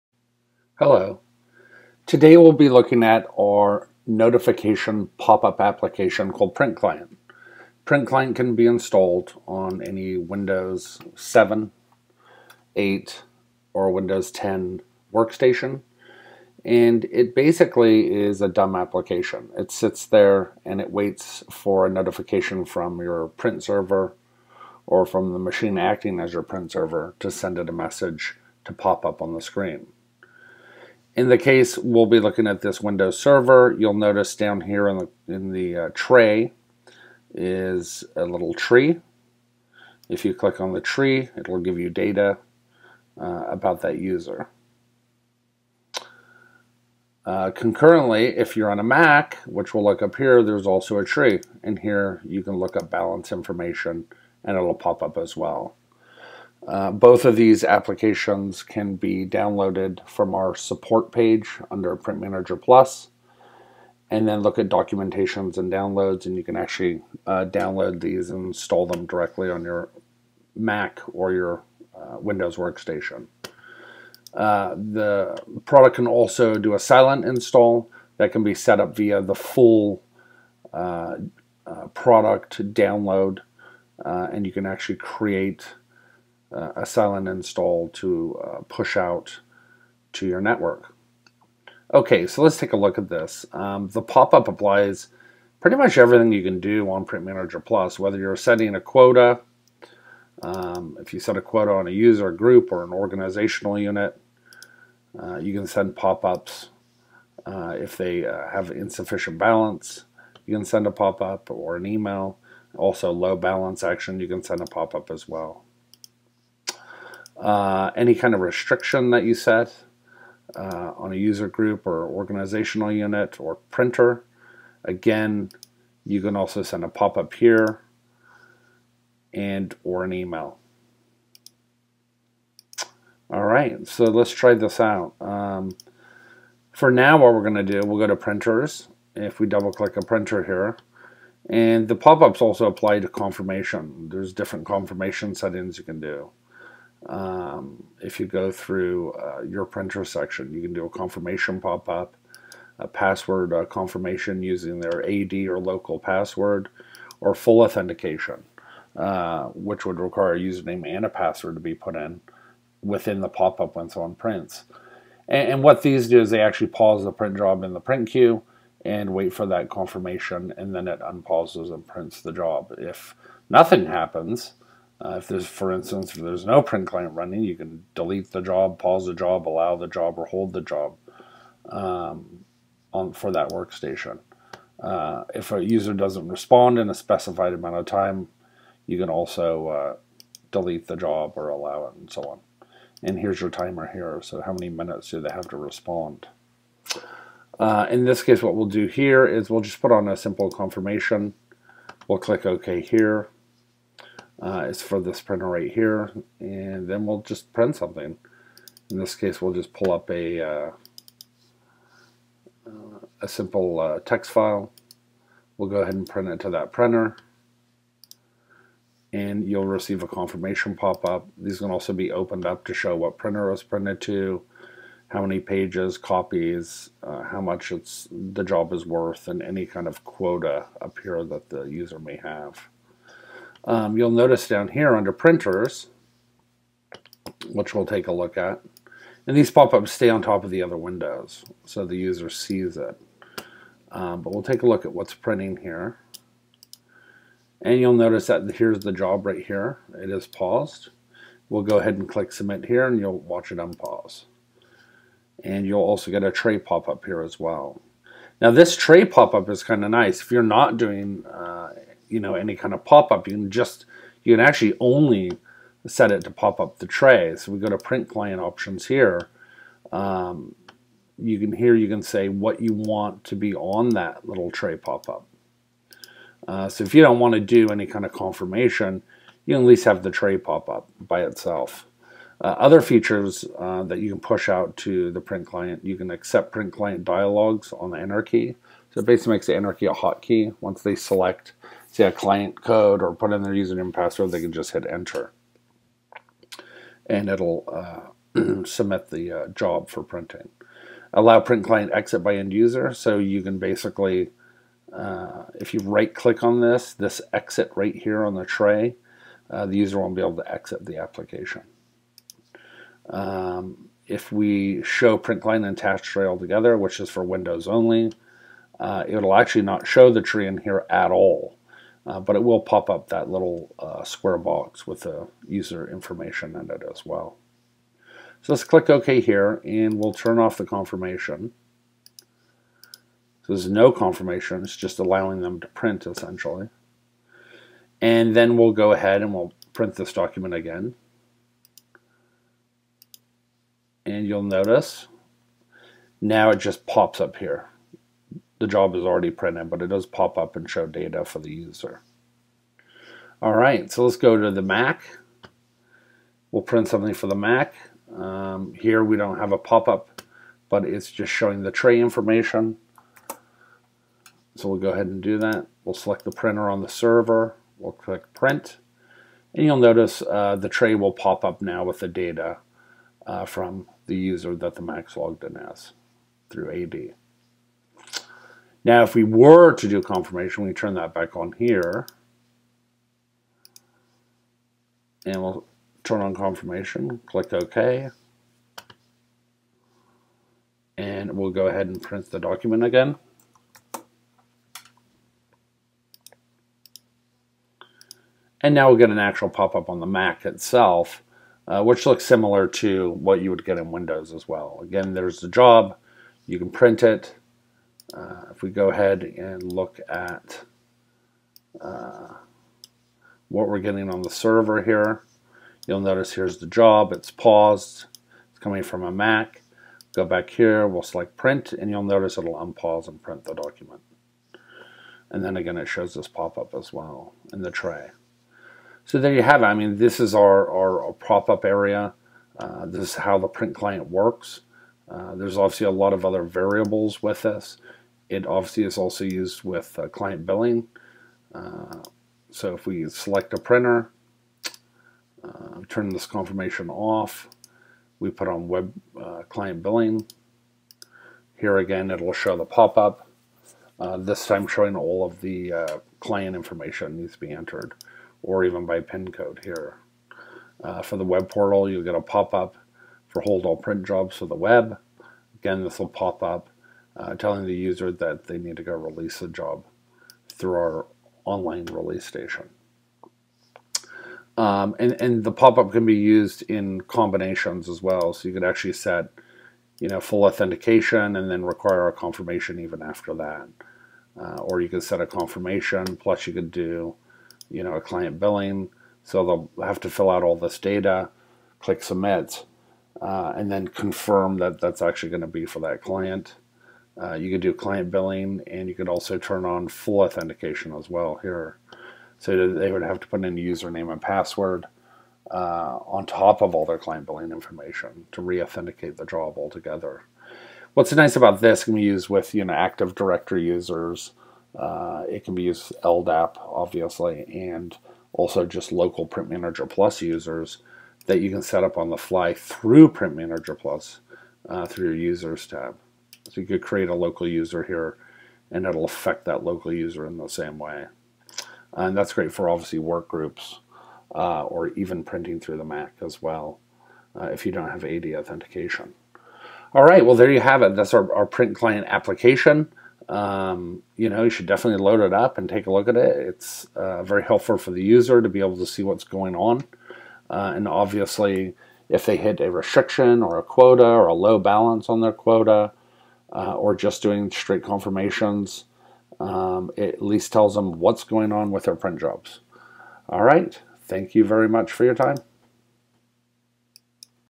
Print Client Pop Up Notification Demonstration
General Demonstration of the Print Client Notification Pop-Up application settings and features for Windows and MAC.